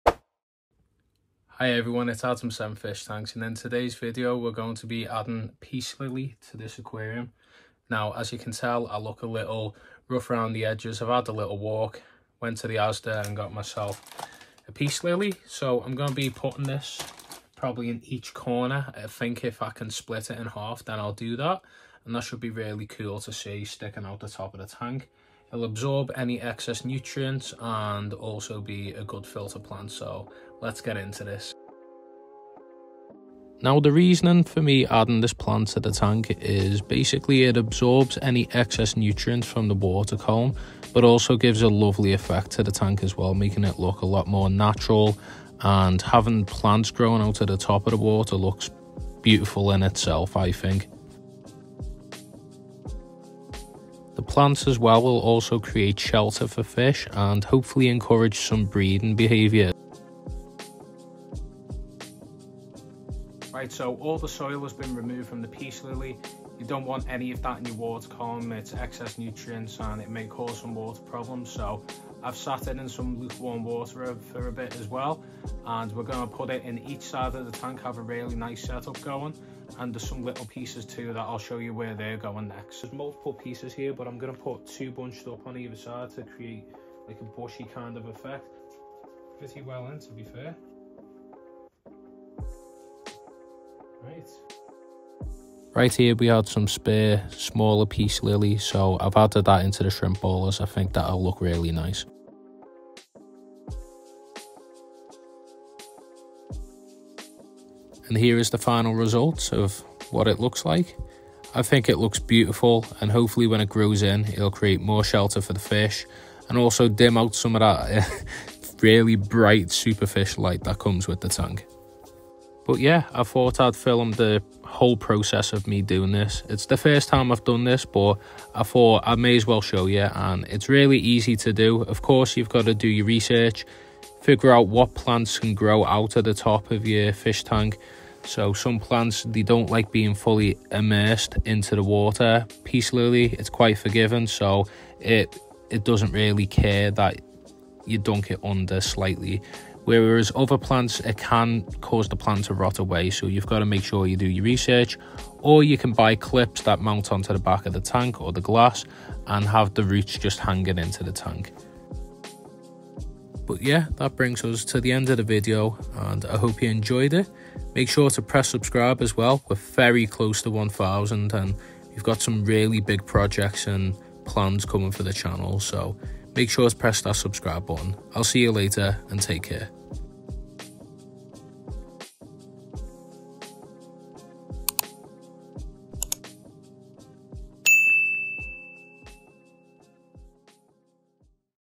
How To Add A House Plant To An Aquarium (Easy)
In this video I show how to add a house plant to an aquarium, this shows the process I go through as it’s my first time doing this!